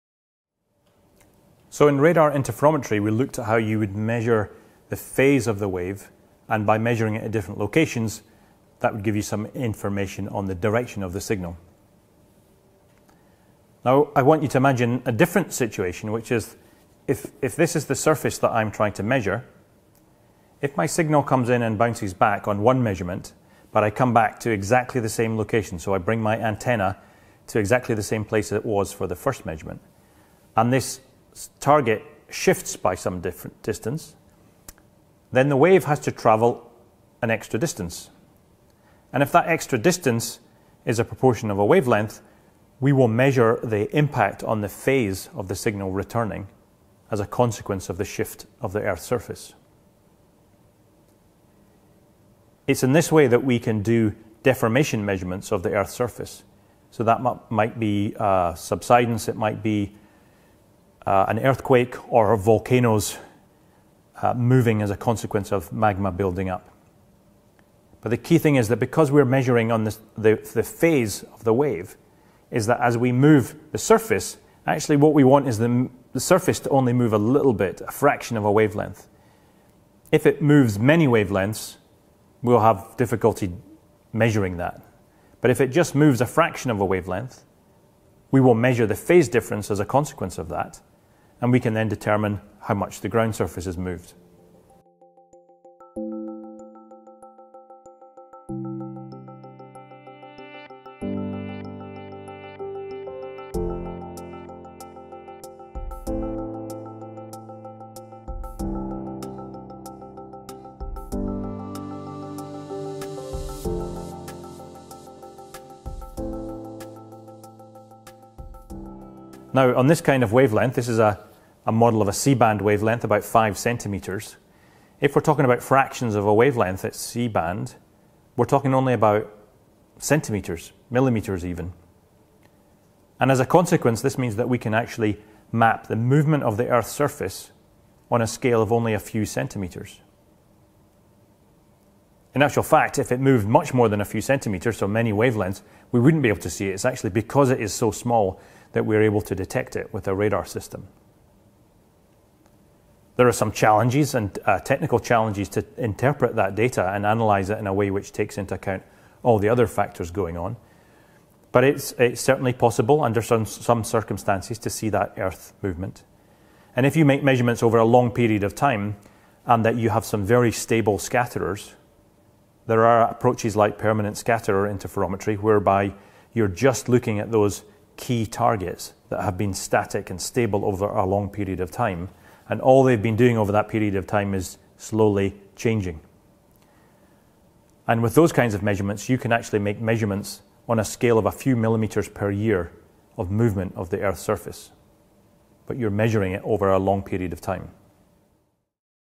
ESA Echoes in Space - Hazard: Introduction to Subsidence Monitoring
Prof. Iain Woodhouse explains the basics of subsidence monitoring with radar.